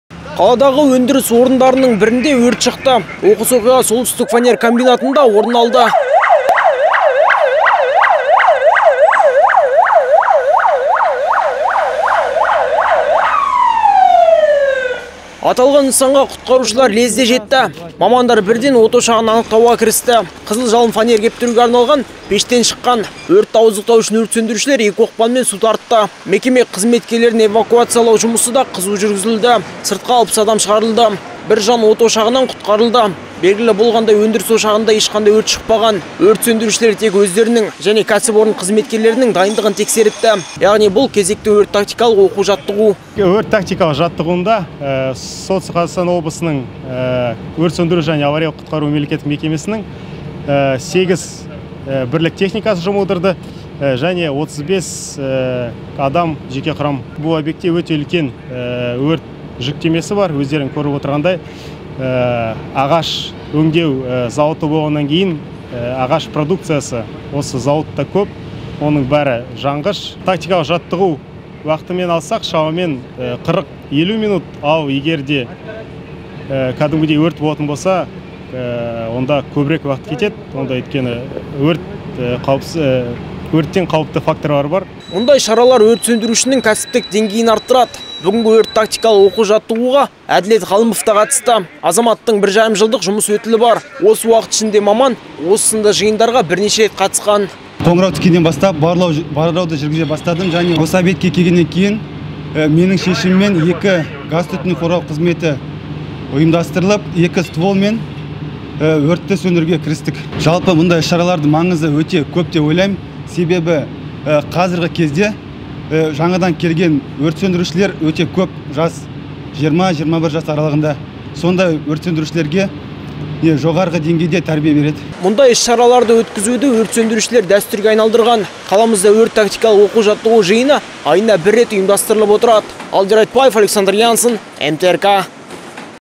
Кезекті өрт-тактикалық оқу-жаттығулар жиыны өтті
Қаладағы өндіріс орындарының бірінде өрт шықты. Оқыс оқиға "Солтүстік фанер комбинатында" орын алды.